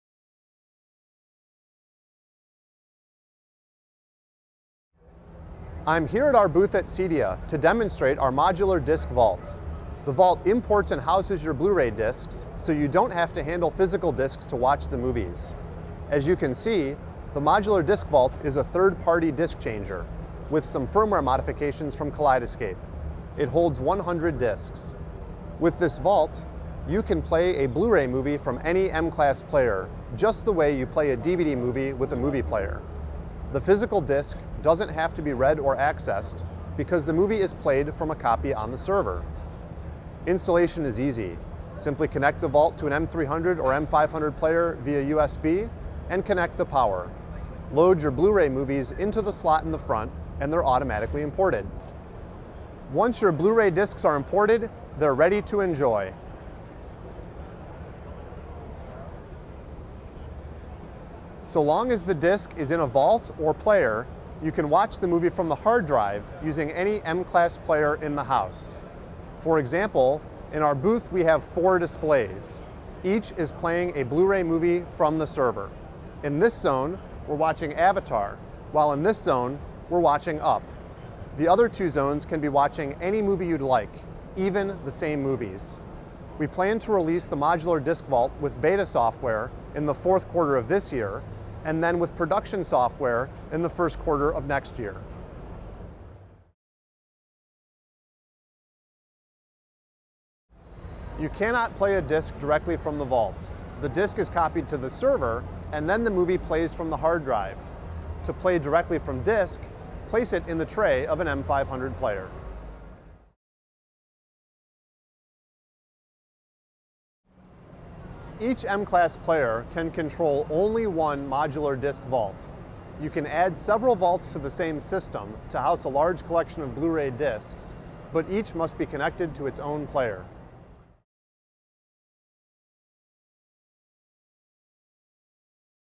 Kaleidescape Modular Disc Vault at CEDIA EXPO 2010
Demonstration of the Modular Disc Vault, which enables Blu-ray movies to be played from a Kaleidescape movie server without handling a disc.